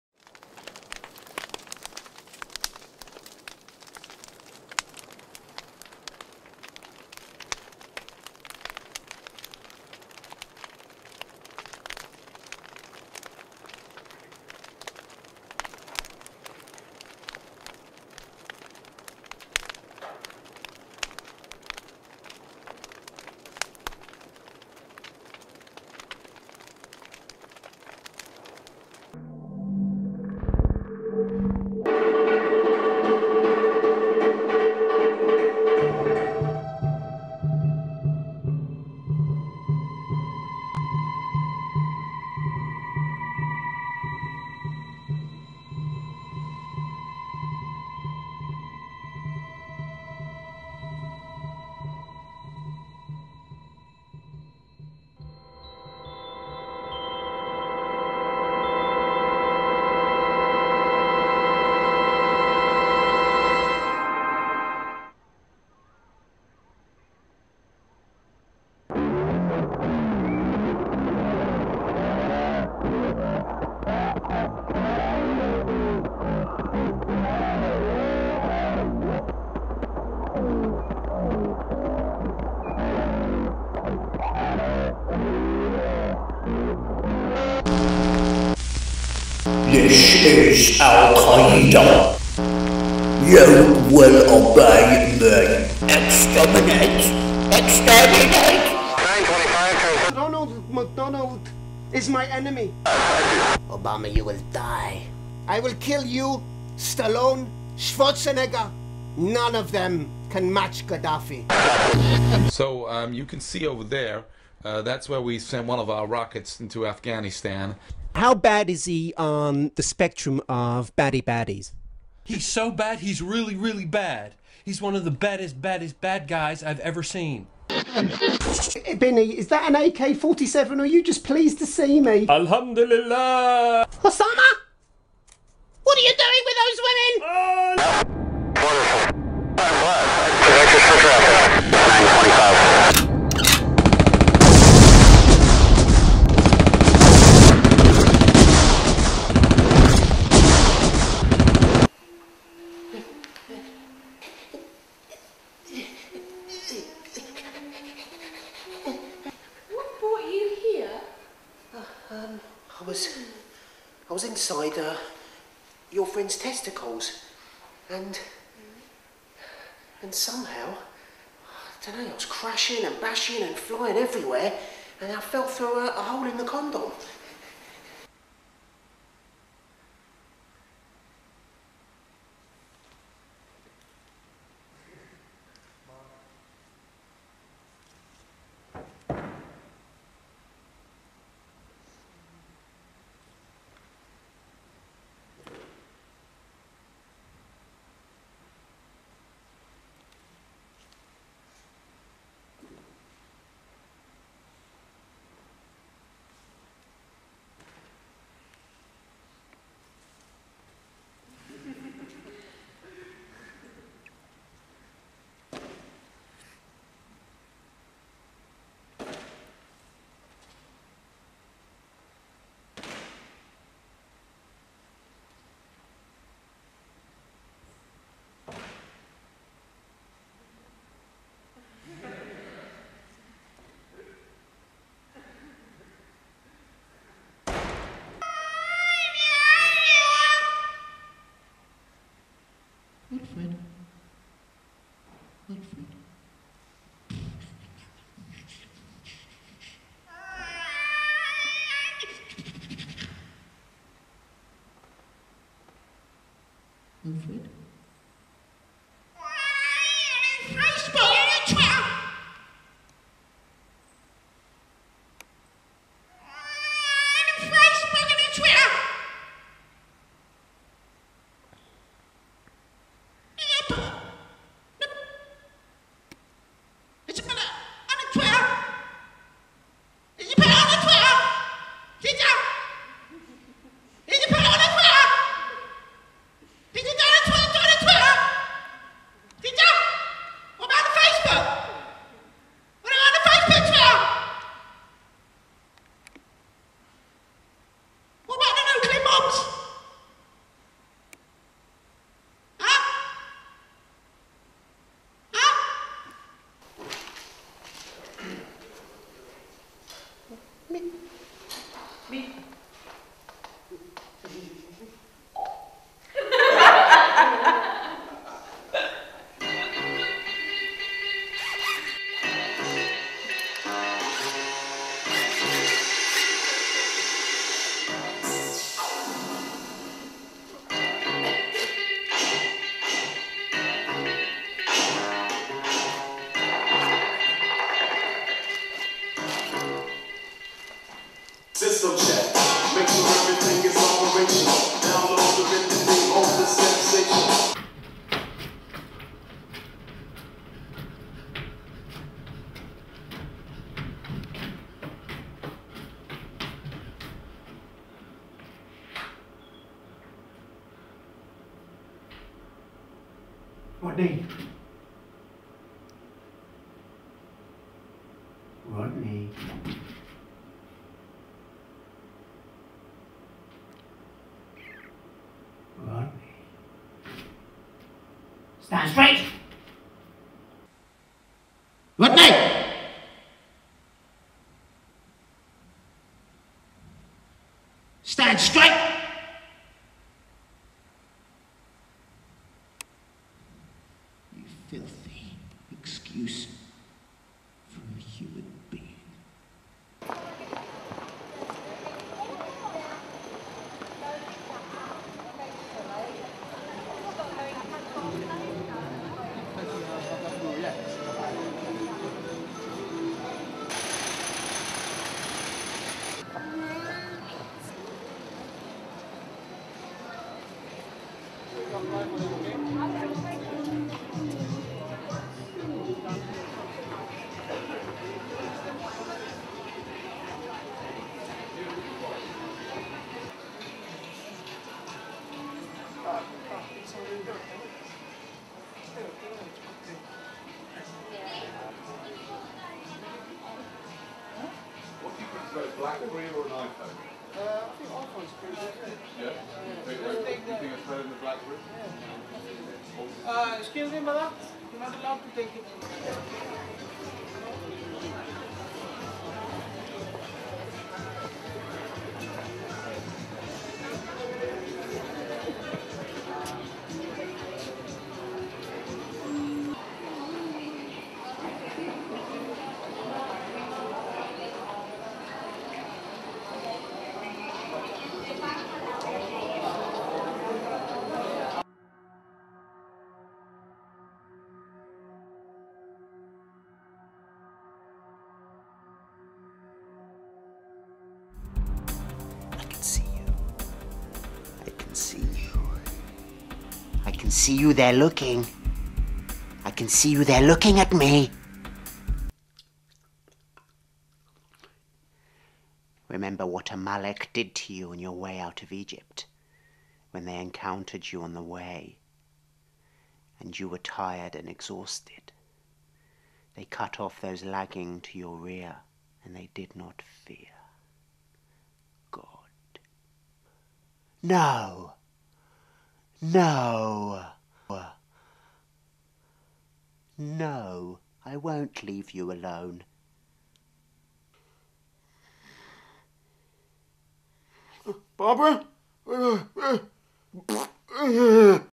MAYHEM acting reel ALEPH
elliott tucker acting reel

feat. mayhem at Euro Disney, CIA spoof, therapy puppets dir. Leah Bartal, classic Rodney the Nebbuch live, Ronnie loose in Redbridge w. Danny Shine and Amalek strikes.